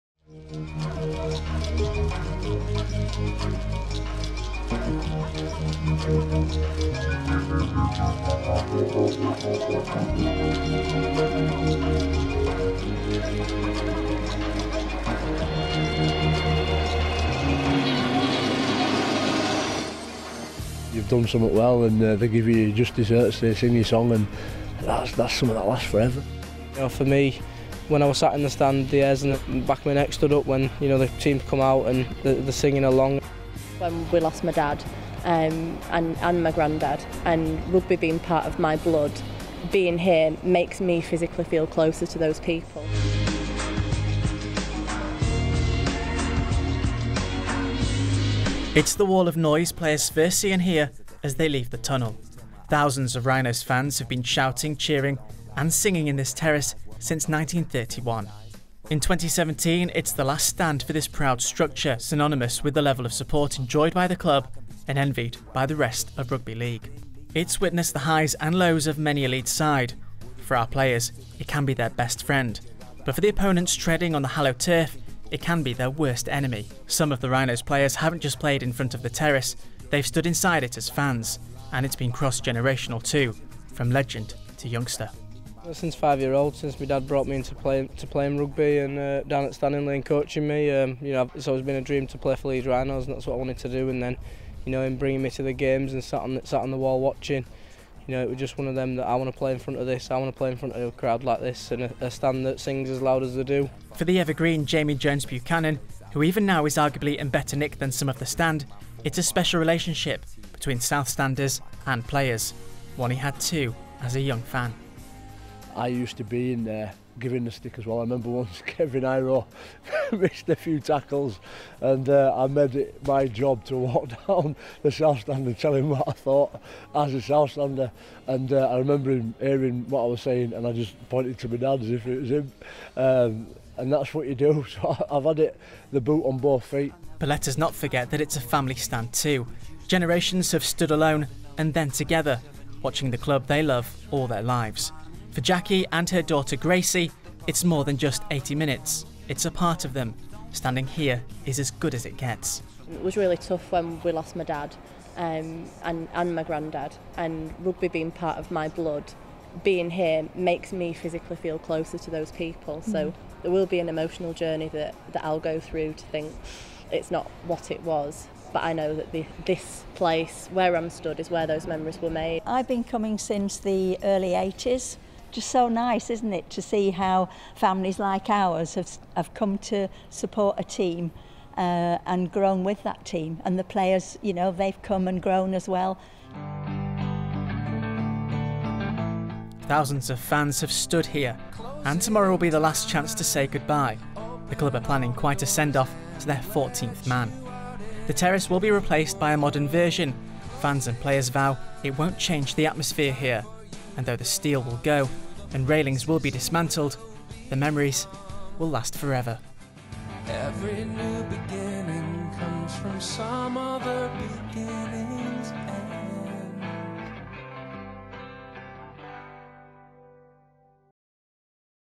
Headingley Stadium's South Stand
Headingley Stadium, home to Leeds Rhinos has recently been redeveloped.

But before it was updated, it was home to an old Terrace many fans and players called home.

This was my story on the structure loved by many before it was torn down in 2017.